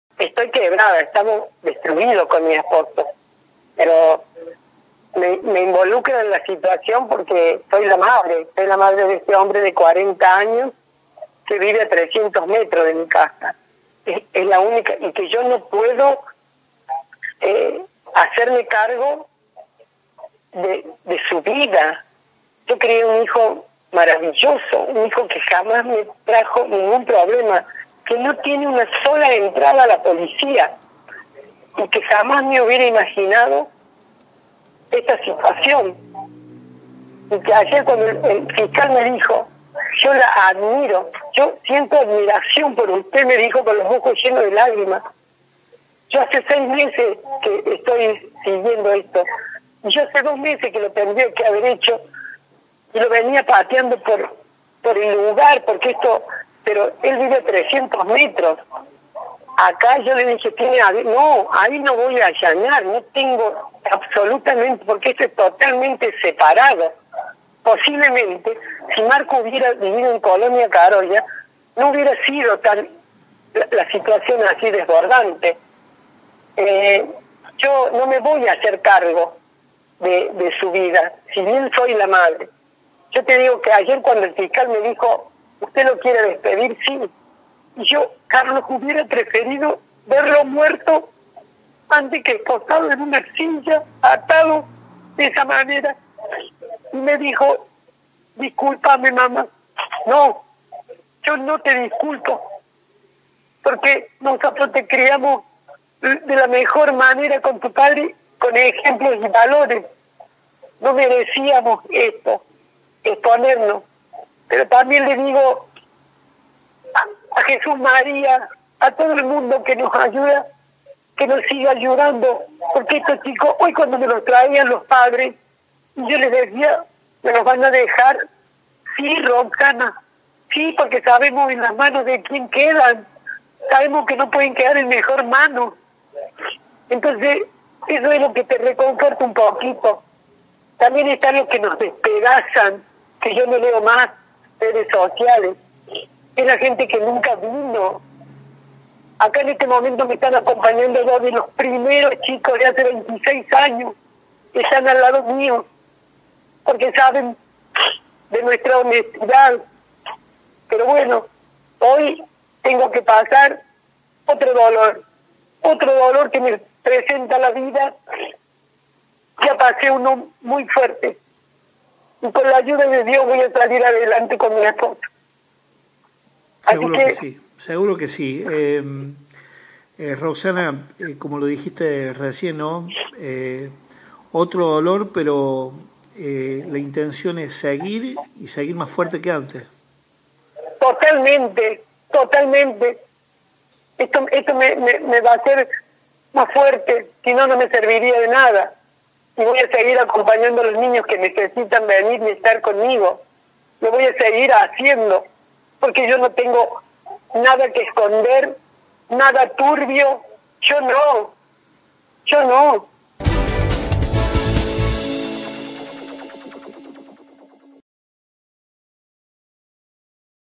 Habló la fundadora de "La Casa de Matías", tras la detención de su hijo por presunta venta de drogas
La fundadora de "La Casa de Matías", Roxana Toranzo, se mostró muy dolida por lo ocurrido con su hijo, quien fue detenido en las últimas horas por la Fuerza Policial Antinarcotráfico por presunta venta de estupefacientes en Jesús María. Aclaró que él vive a 300 metros de su casa y que nunca imaginó que iba a estar involucrado en un hecho así. Agradeció el apoyo recibido.

🎥 Roxana Toranzo, fundadora de La Casa de Matías.